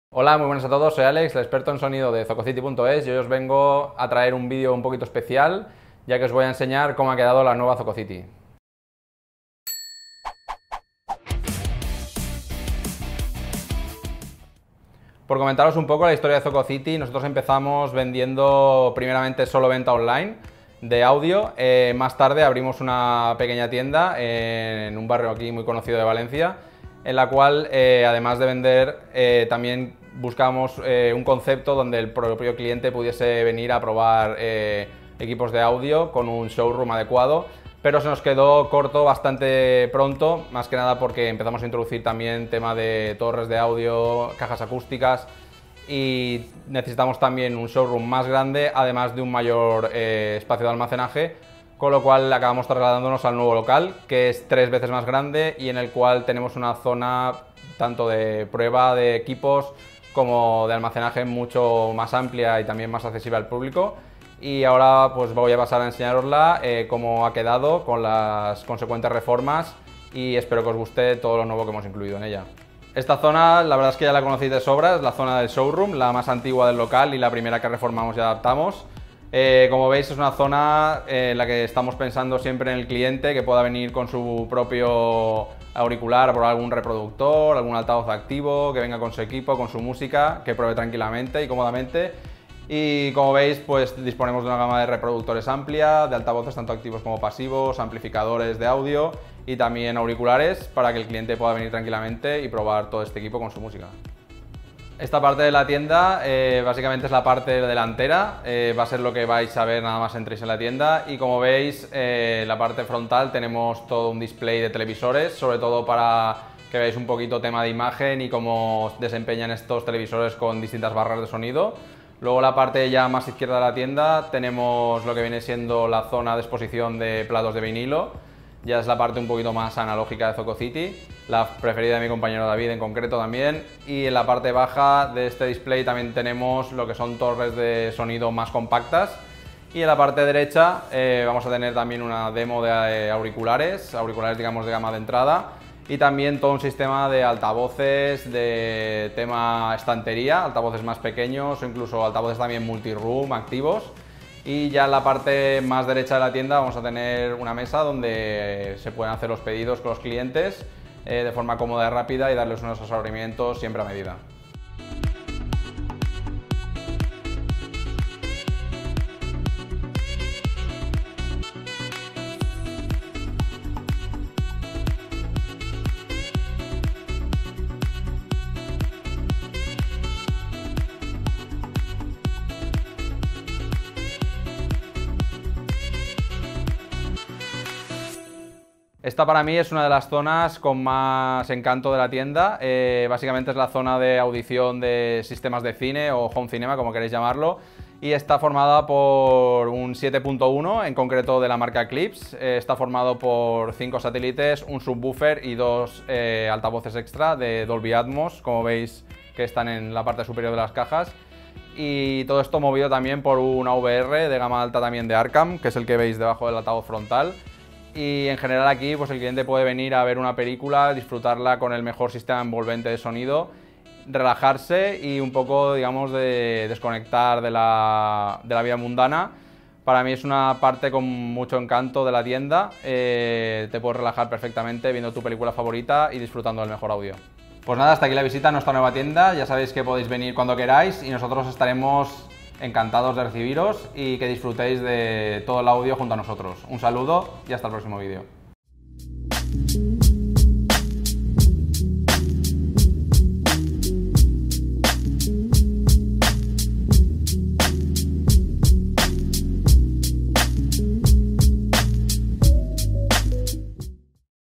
Seguimos creciendo: home cinema, televisores y más sonido
Durante el último mes,  nuestra tienda física y web han experimentado cambios muy importantes.

Seguimos creciendo y ampliamos el catálogo de sonido, home cinema e introducimos televisores, para potenciar lo mejor del audio doméstico y profesional.

Si quieres ver como son nuestras nuevas instalaciones, te invitamos a nuestro tour.